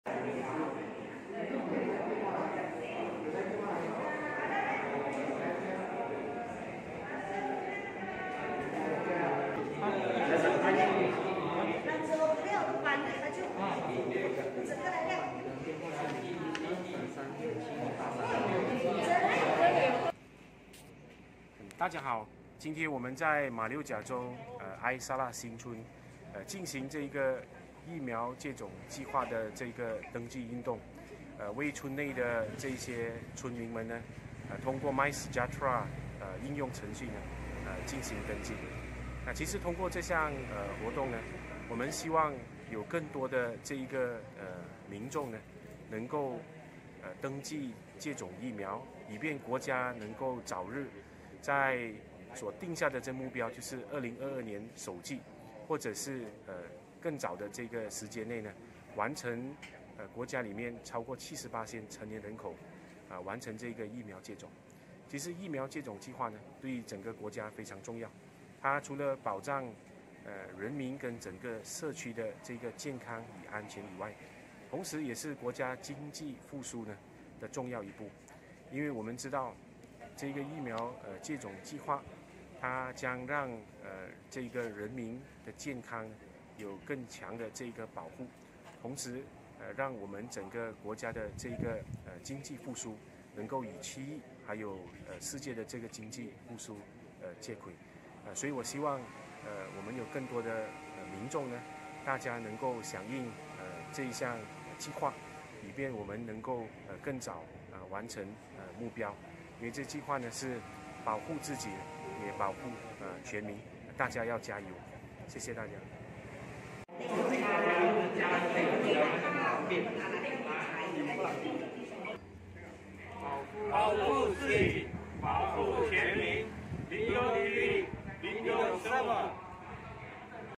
马华办觉醒运动呼吁 响应疫苗接种计划
（马六甲13日讯）保护自己，保护全民，一起响应疫苗接种计划吧！

（ 视频：陈春汉 ）